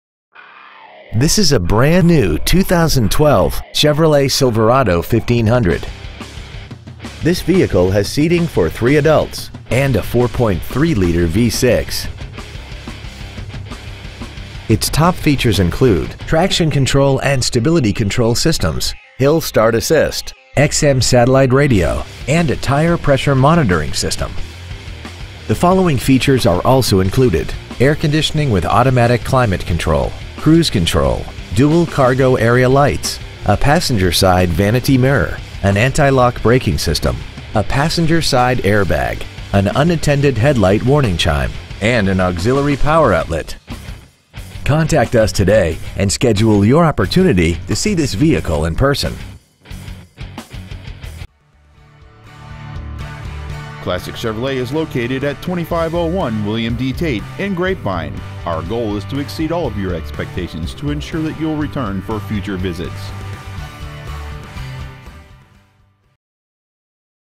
2012 Chevrolet Silverado 1500 Dallas TX
Year : 2012
 Make : Chevrolet
 Model : Silverado 1500
 Engine : Gas V6 4.3L/262
 Trans . : Automatic
 Exterior : Summit White

 2012 Chevrolet Silverado 1500 Grapevine TX